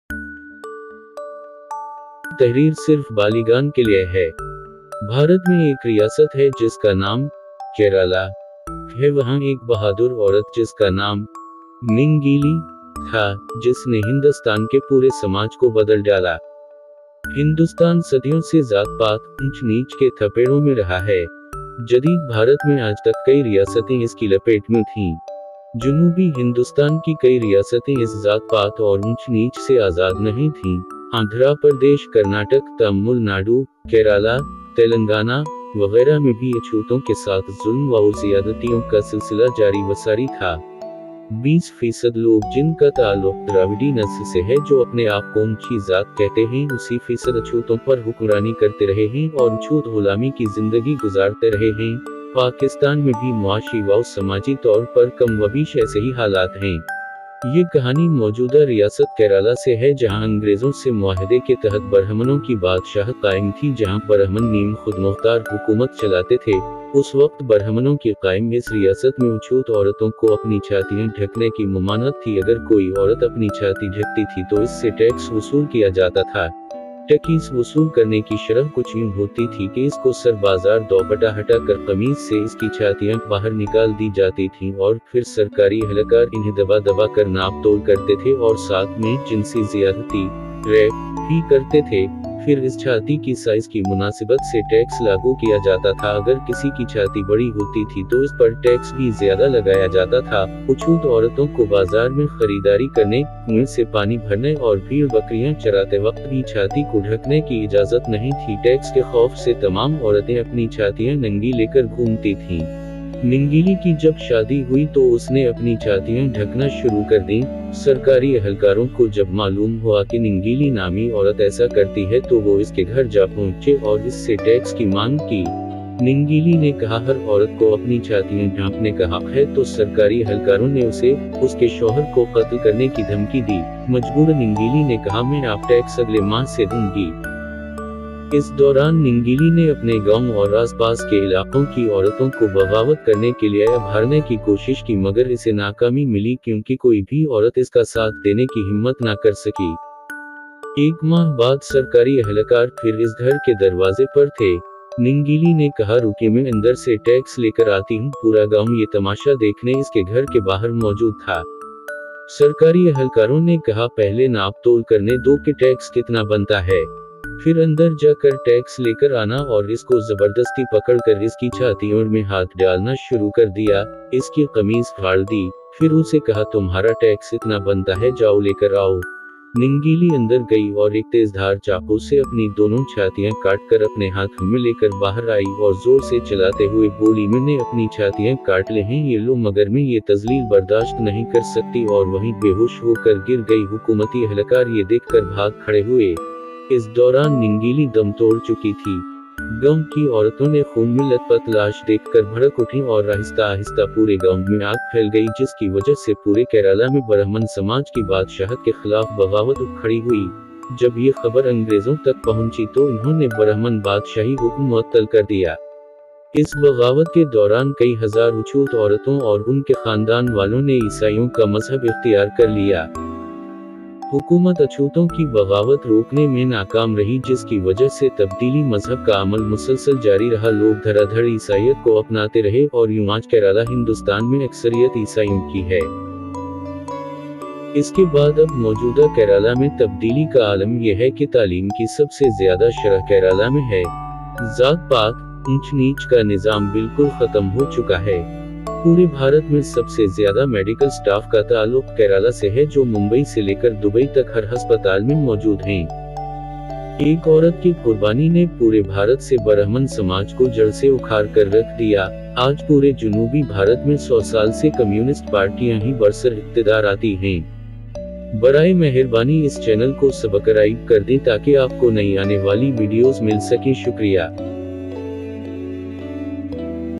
Breast Tax | The Nangeli | بھارت میں چھاتی کے مطابق ٹیکس لاگو کیا جاتا تھا | By Sajjadaliwariss
This story about Breast Tax | The Nangeli | بھارت میں چھاتی کے مطابق ٹیکس لاگو کیا جاتا تھا